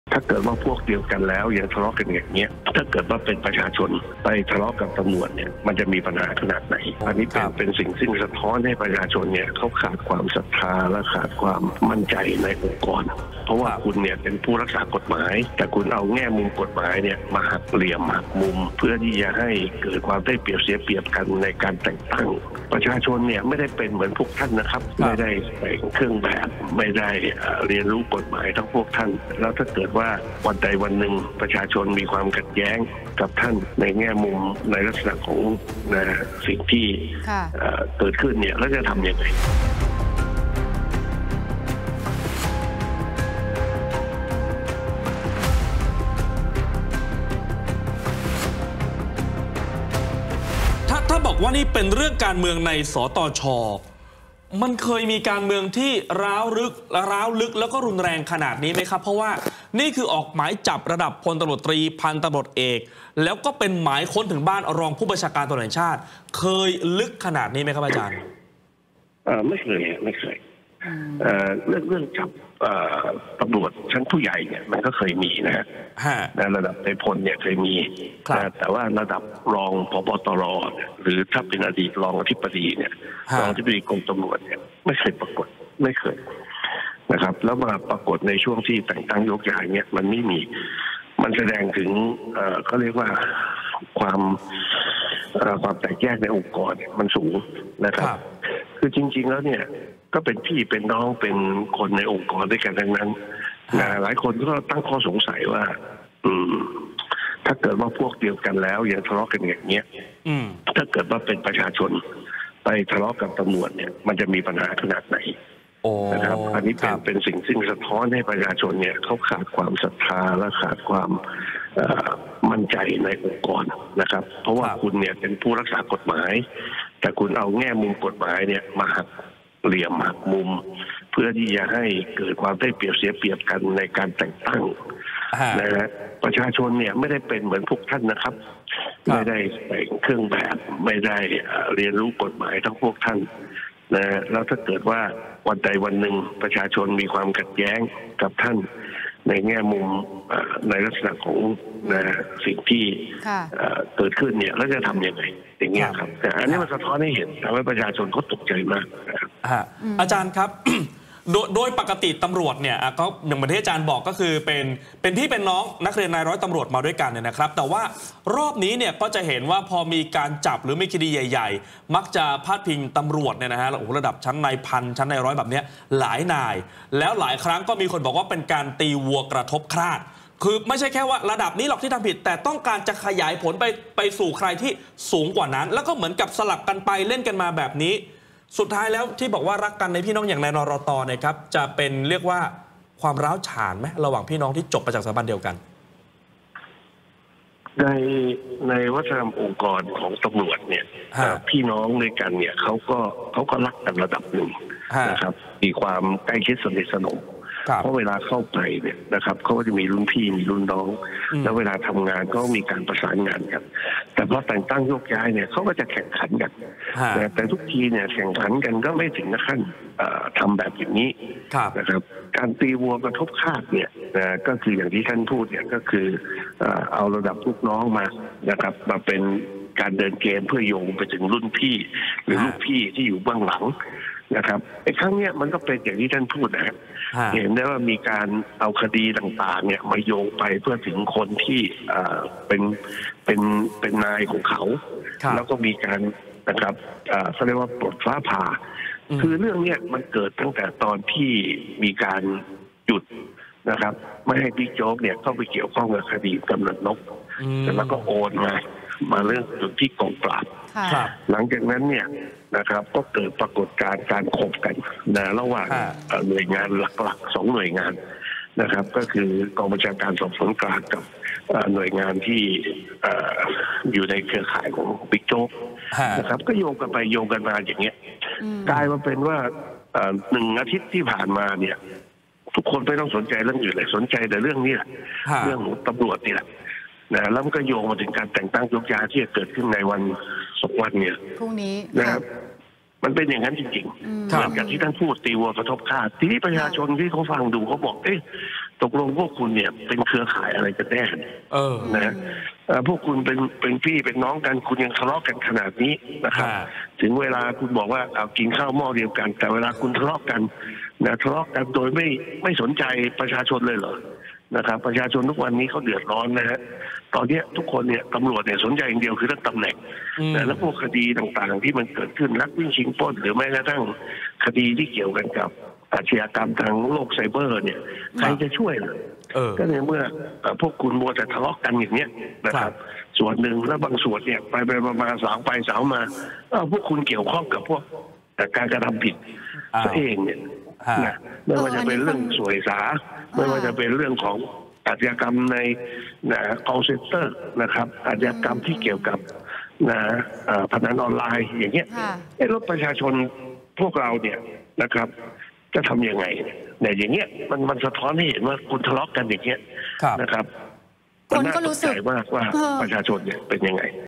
การเมืองใน ตร.แรง!
"ขัดแย้งกันเองยังขนาดนี้ มีปัญหากับประชาชน จะขนาดไหน?"

อดีตนายตำรวจ อาจารย์นักกฎหมาย มอง การเมืองใน ตร.แรง!